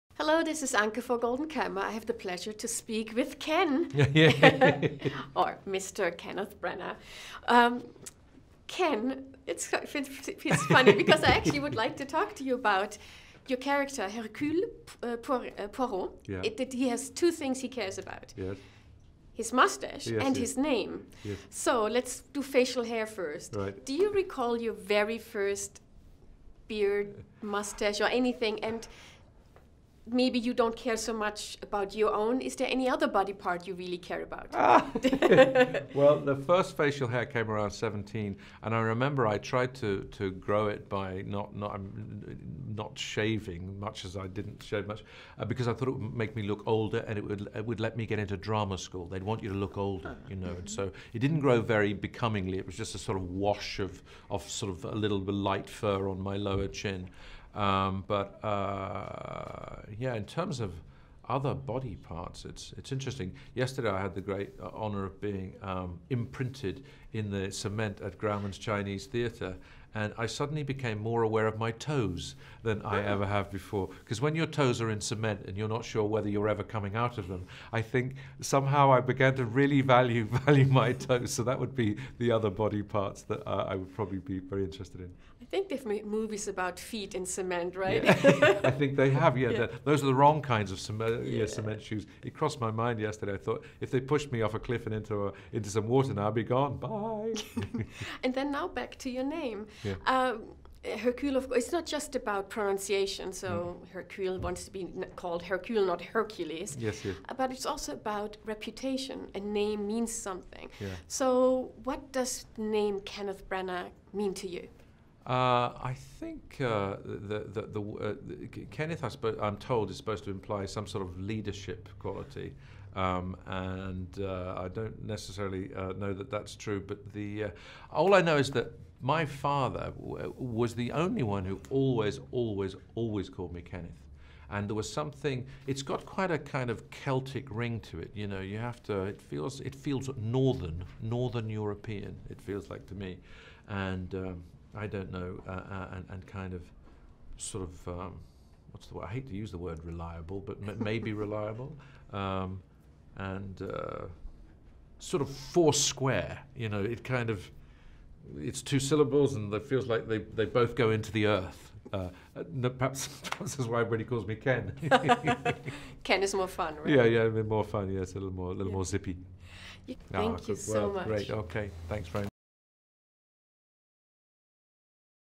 Kenneth Branagh zur Neuauflage von "Mord im Orient-Express"
Beim Reboot des Agatha-Christie-Klassikers "Mord im Orient-Express" (ab 9. November im Kino) führte Kenneth Branagh Regie und schlüpfte in die Rolle von Meisterdetektiv Hercule Poirot, dem zwei Dinge besonders am Herzen lagen: sein Bart und sein guter Name. Kenneth Branagh auch...?